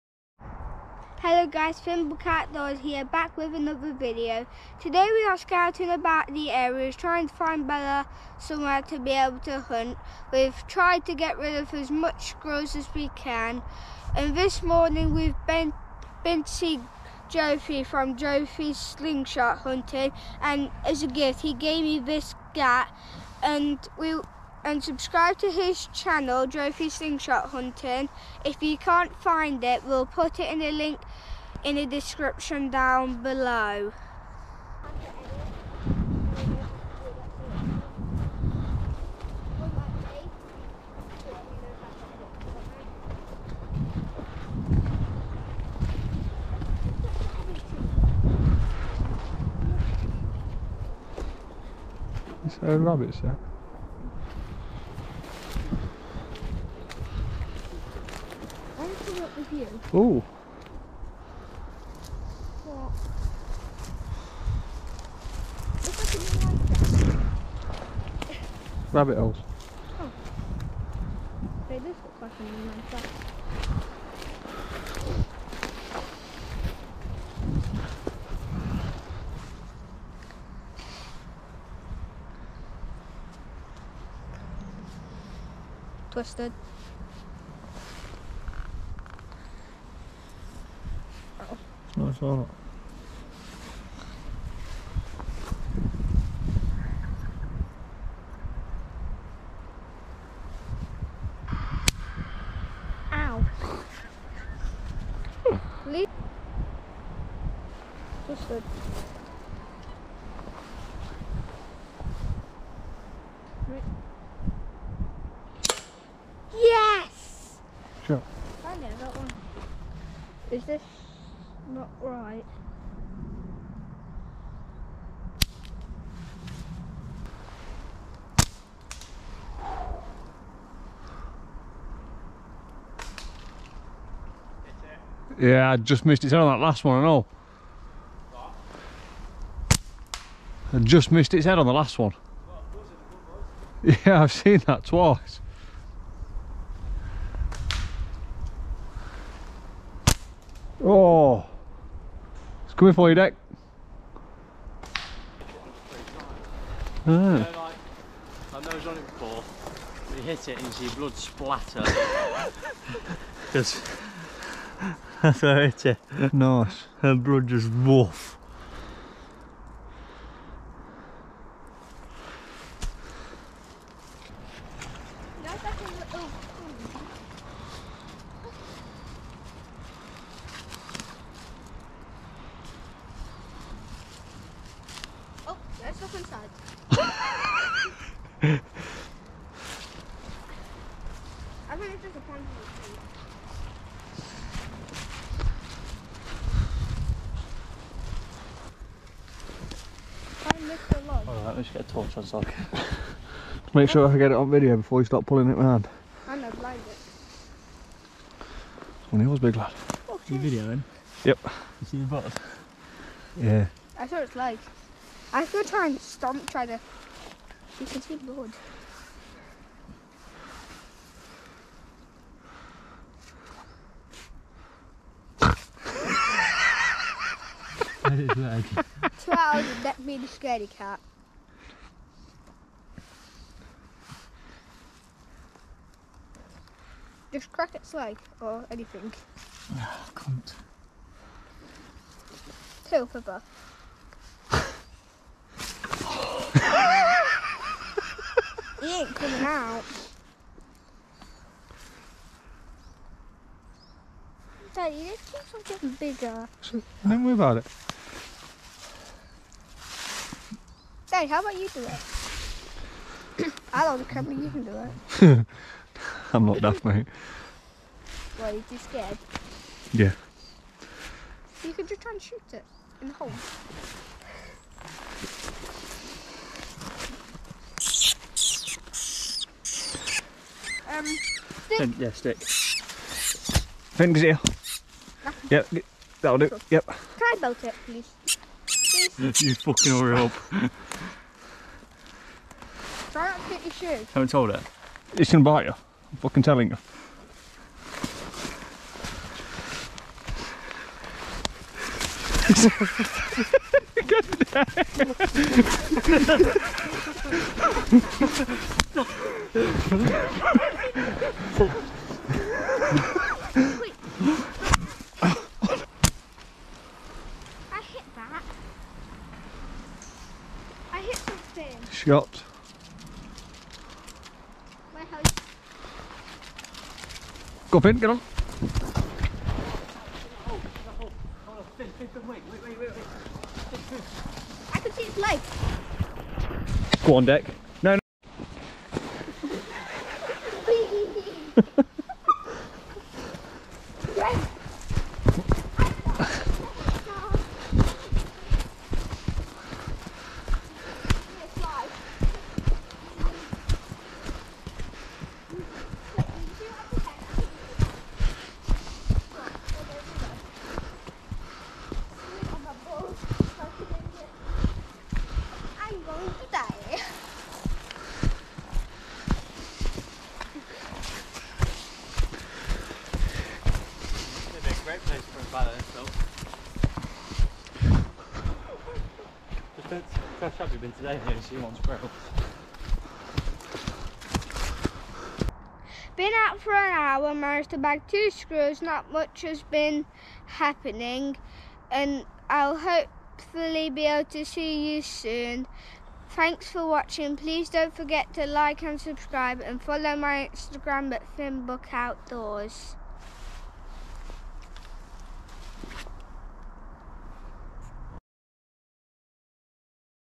squirrel shooting with catapult
An afternoon on the squirrels with my new ttf slingshot that @Joefeslingshot very kingly gave me 👍